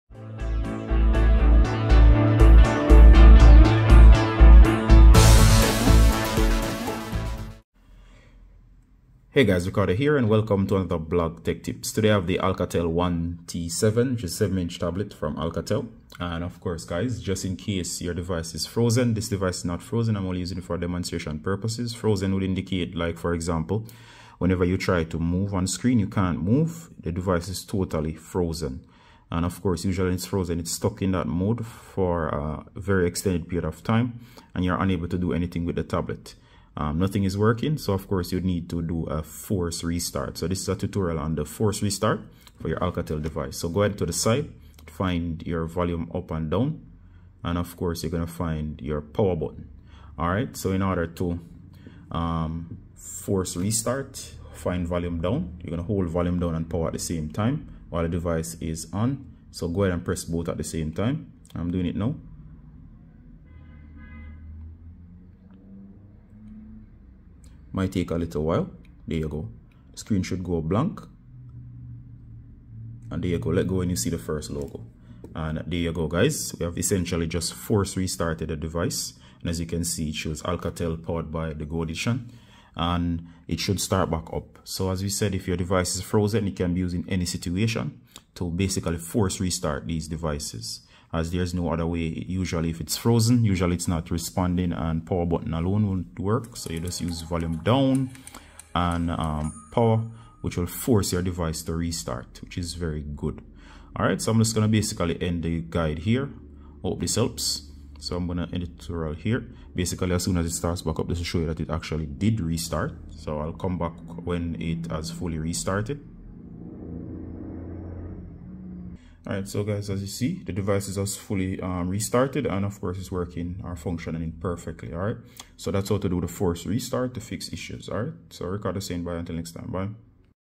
Alcatel 1T7 Force Restart
Alcatel 1T7 Force Restart your device if it is frozen and unresponsive. Make your device responsive again.